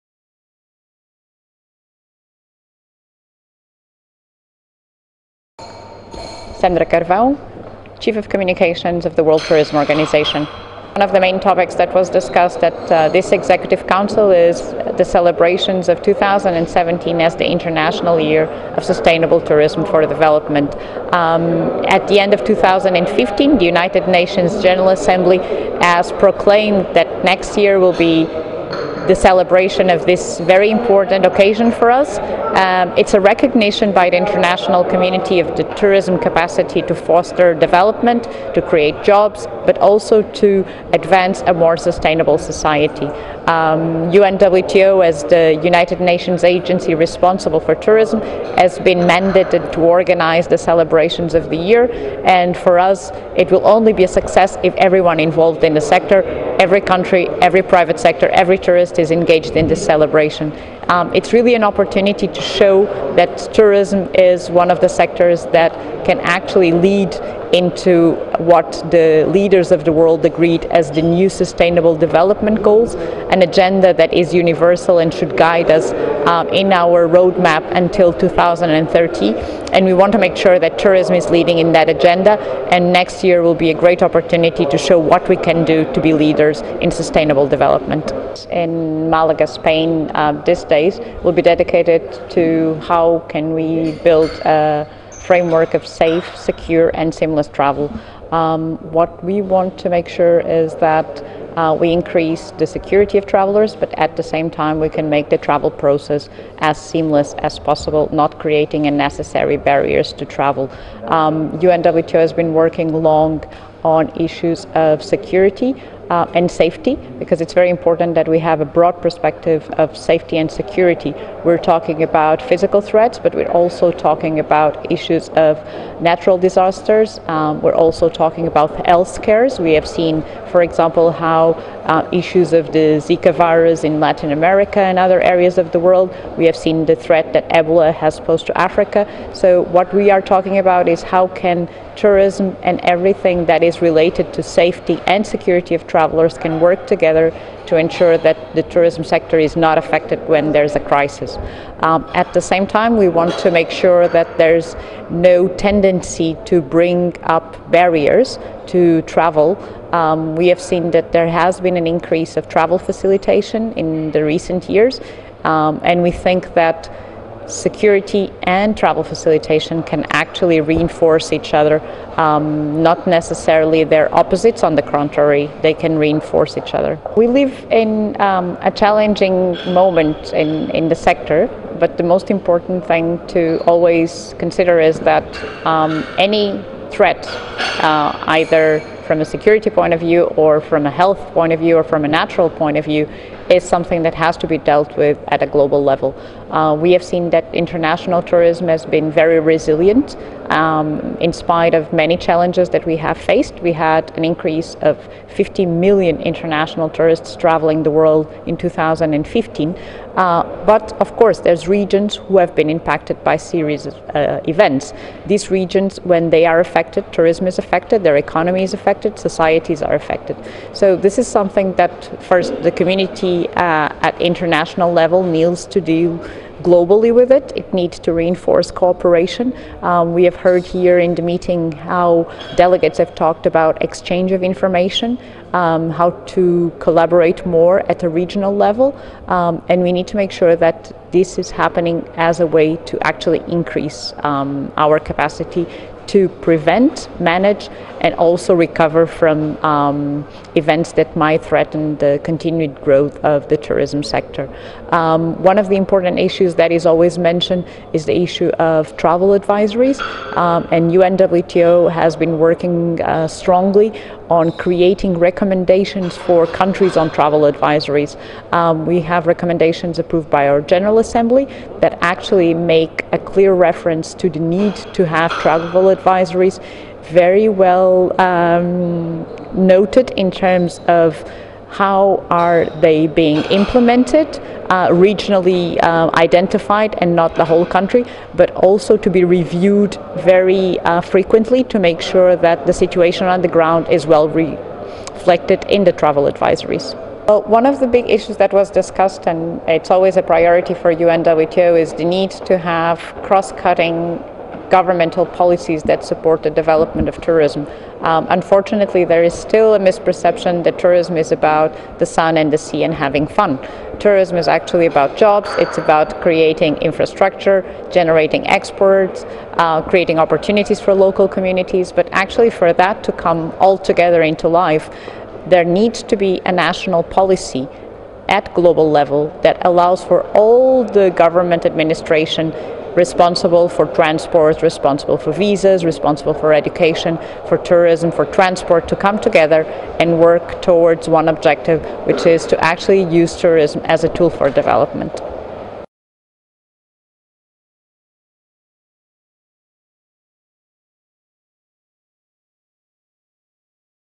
UNWTO Communications Chief speaks to Touristswatch
The United Nations World Tourism Organization (UNWTO) Chief of Commucations, Sandra Carvao speak exclusively to Touristswatch on the sidelines of the 103rd UNWTO Executive Council  Meeting in Malaga, Spain